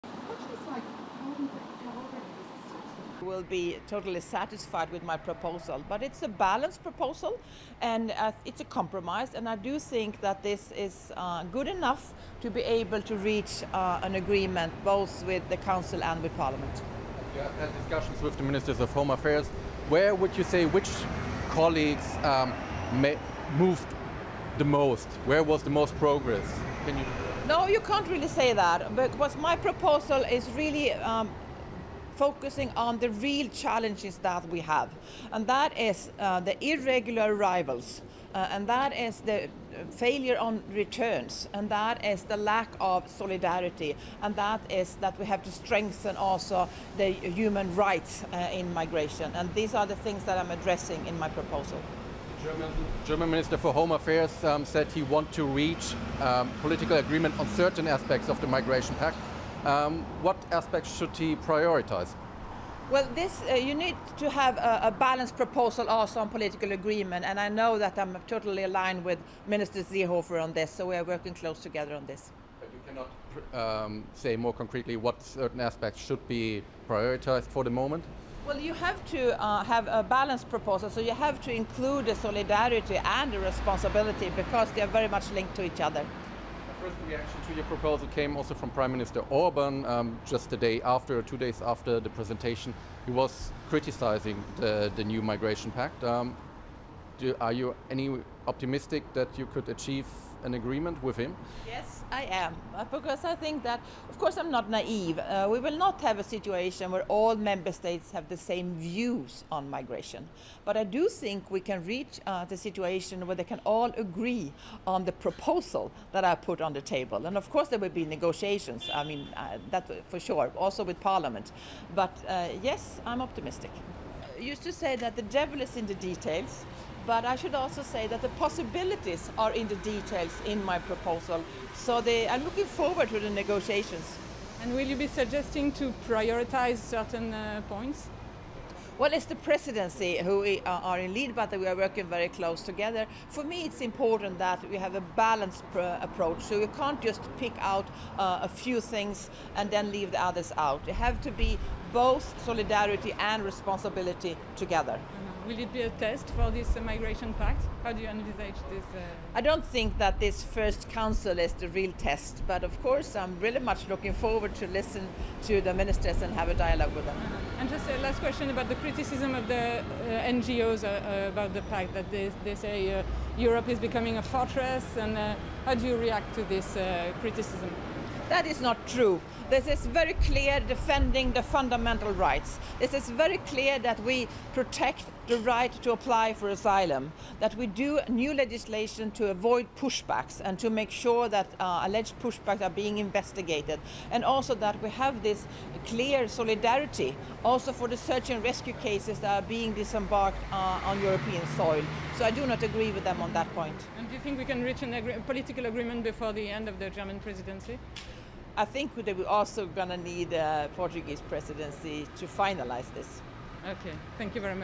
Ylva Johansson eudebates MigrationEU proposals with EU Interior Ministers
I am totally satisfied with my proposal. It's a balanced proposal, it's a compromise, and I do think that this is good enough to be able to reach an agreement both with the Council and the Parliament.

Which countries, which colleagues did the most progress?

No you can't really say that. My proposal is really focusing on the real challenges that we have: the irregular arrivals, the failure on returns, the lack of solidarity, and the fact that we have to strengthen the human rights in migration. And these are the topics I am addressing in my proposal.

Which aspects of the migration pact should be prioritized?

Well you need to have a balanced proposal on some political agreements and I know that I am totally aligning with Minister Seehofer on this. So you have to include the solidarity and the responsibility because they are very much linked to each other.

Critics by Orbán on migration pact?

Of course I am not naive, it's difficult to have a situation where all the member states share the same views on migration. But I do think we can reach the situation where they can all agree on the proposal that I've put on the table. For sure there will be negotiations and this also with the Parliament but yes I am optimistic!

I should also say that the possibilities are in the details in my proposal so I am looking forward with the negotiations.

Priorities?

Well it is the German Presidency that has the lead but we are working very close together. For me it's important to have a balanced approach so you cannot just pick out a few things. Solidarity and responsibility need to be both together.

Will it be a test for this Migration Pact?

I don't think that this first Council is the real test but of course I am really much looking forward to listen to the Ministers and to have a dialogue with them.

Criticism of NGOs on this Pact: "EU is becoming a forteresse"?

That is not true! It is very clear that this Pact is defending the fundamental rights, that it protects the right to apply for asylum, that we do new legislation to avoid push-backs and to make sure alleged push-backs are being investigated. And also that we have this clear solidarity also for the search and rescue cases that have been disembarked on European soil so I do not agree with them on that point.

Do you think we can reach a political agreement by the end of the German Presidency?

I think we will also need the Portuguese Presidency to finalise this.